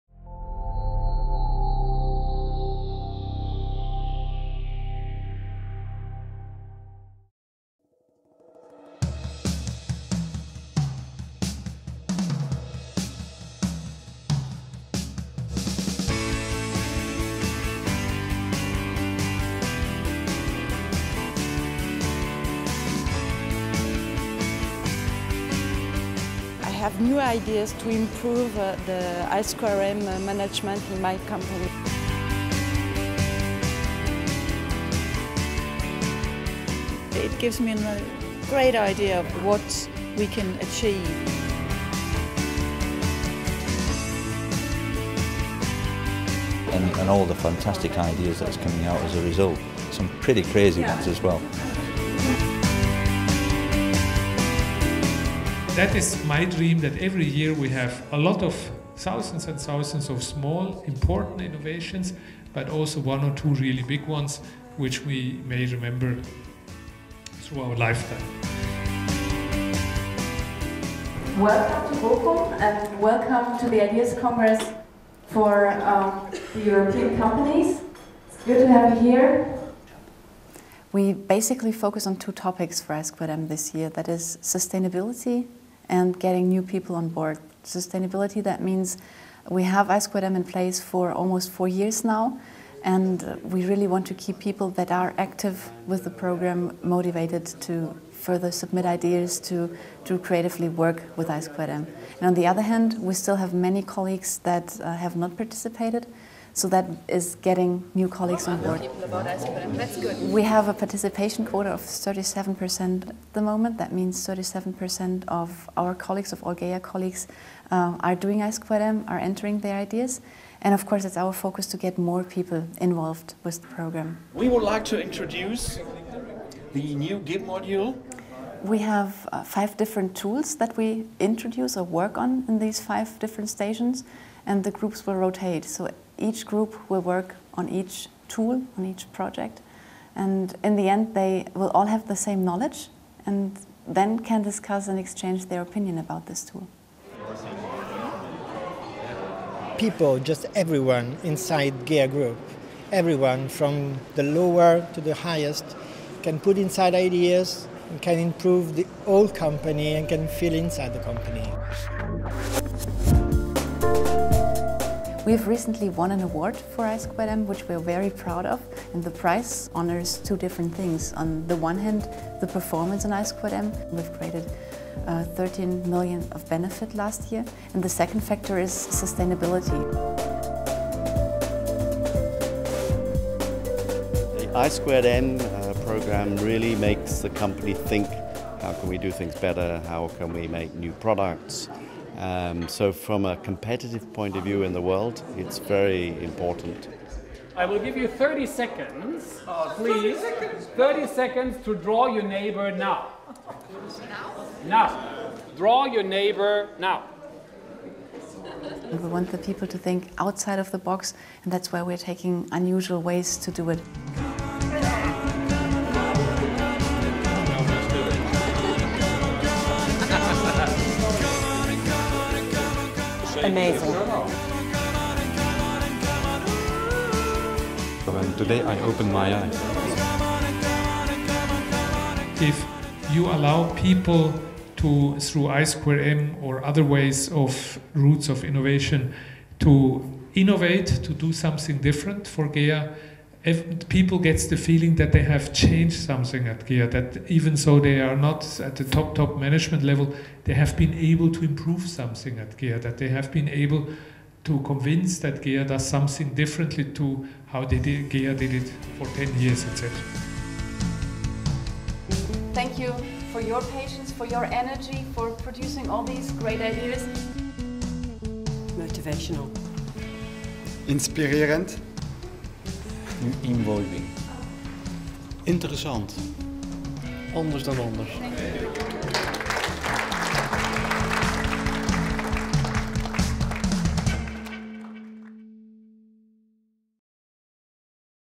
GEA Group - i²m: Share your ideas (English)
In order to improve our products and processes in all areas of our business (in production and administration), and thereby sustainably increasing the corporation's value, we invest in the experience of our employees. After all, they are the absolute experts in their areas of responsibility. The GEA Group supports and uses the innovative drive of each individual through "i²m", the group-wide Ideas & Improvement Management.

Employees all around the world are invited to participate in i²m and have the chance to help shape their personal working environment and thereby optimize processes or make their work easier, as well as contributing to health and safety at the workplace. After submission, the ideas are evaluated by the responsible line managers and experts and, when assessed positively, implemented as quickly as possible. And when GEA benefits by a great idea, the employees will benefit as well: GEA gives out cash and non-cash rewards for the savings achieved through ideas.

For 2011, the focus is to sustainably use the improvement potential across all segments and departments of a corporation that operates worldwide and is strongly networked. The fact that we have sustainably established ideas management as part of an open, innovative corporate culture is reflected by the award "Best Ideas Management 2011" in the industry GEA has received from the International Ideas Management Conference organized by the Zentrum Ideenmanagement (ZI) in March 2011.